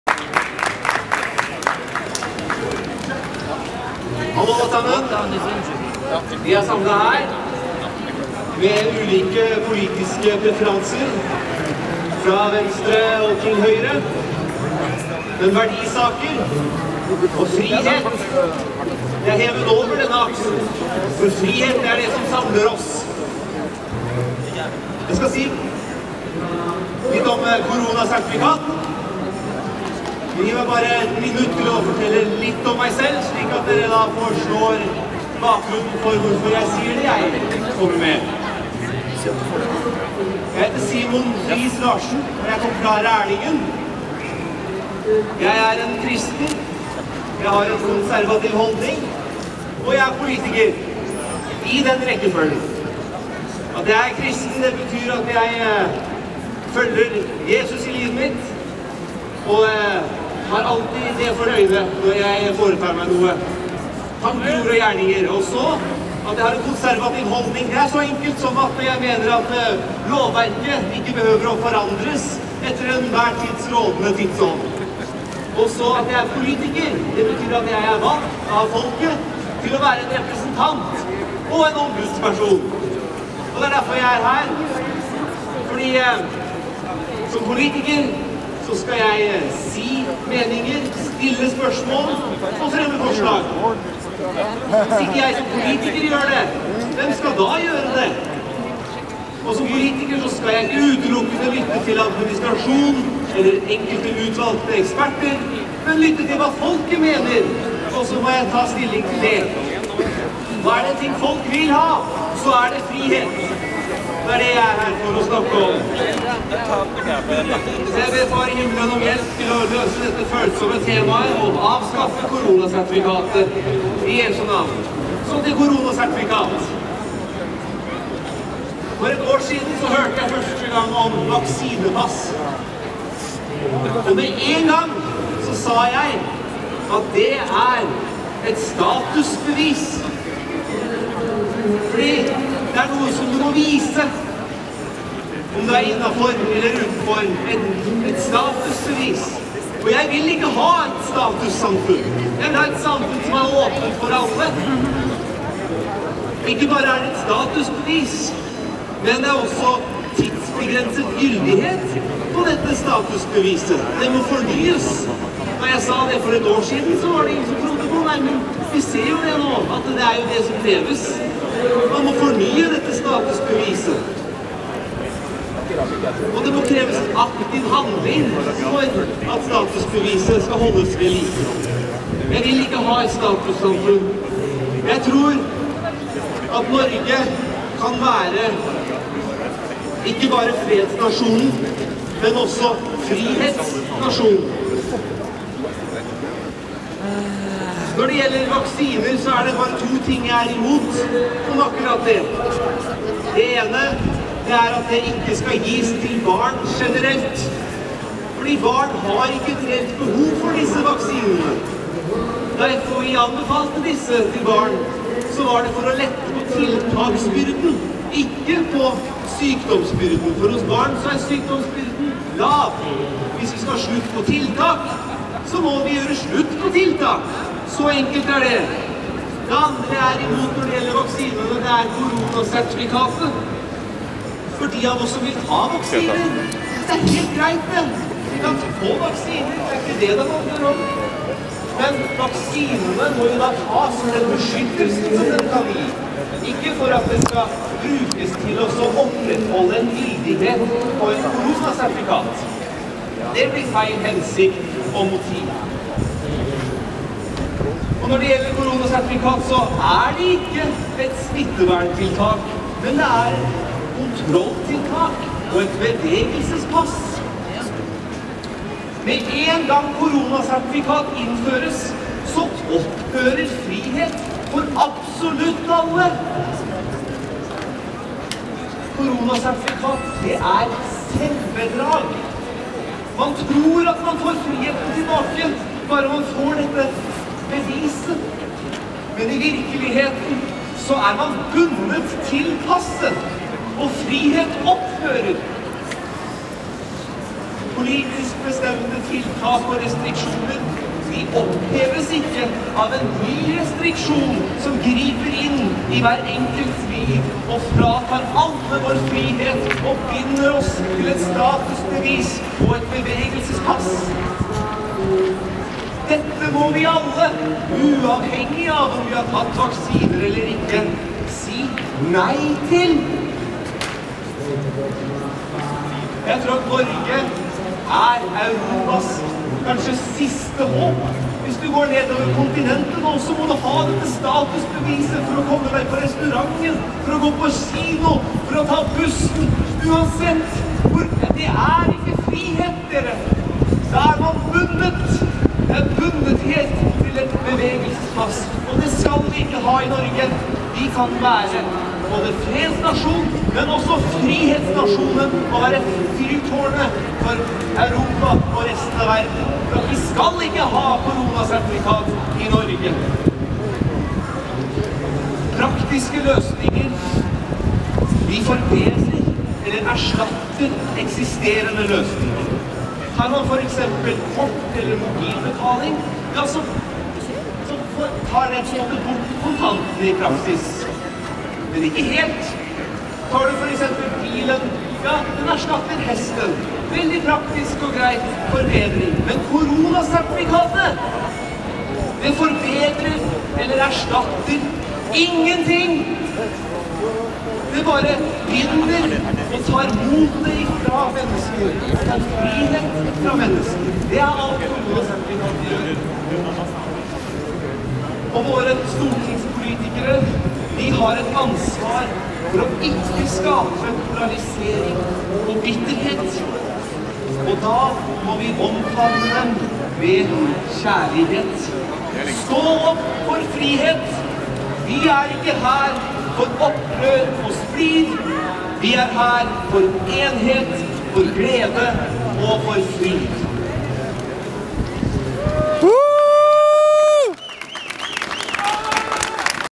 Vi er ikke her for opprør og splid, vi er her for kjærlighet Stå opp for frihet.
Sterke og sanne ord i dag Lørdag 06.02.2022 av Simon Friis Larsen Rærlingen KRF, mot innføring av C.Sertifikat; en tale som kan få en hver til å åpne øynene og dette er en politikers ord.
Min nye favoritt politiker , en som virkelig forteller sannheten og hvor viktig denne saken er. Takk Simon !!
Noen av hans sterke stikkord er;
Med en gang c. sertifikat innføres så opphører all frihet for absolutt Alle, Kvaksinert eller ikke.
Vi må avskaffe C sertifikatet i jesus navn; Ja du hørte riktig, Amen!
Dette status symbolet skal vi ikke ha.

Vi er ikke her for opprør og splid, vi er her for kjærlighet
Stå opp for frihet.
Video av;